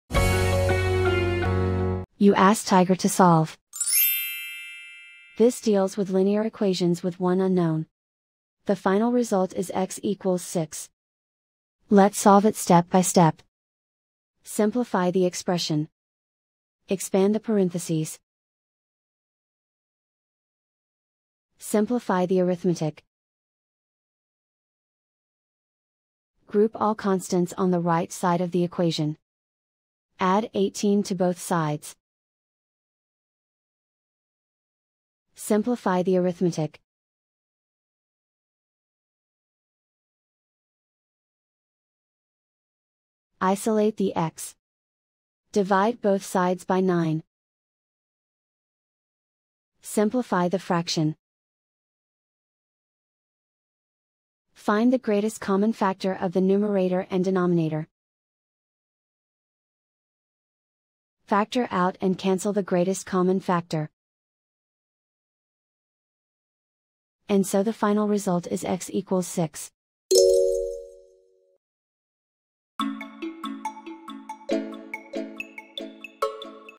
Solve 9(x-2)=36: Linear Equation Video Solution | Tiger Algebra
Struggling with 9(x-2)=36? Watch our step-by-step video on Tiger Algebra to master this linear equation effortlessly.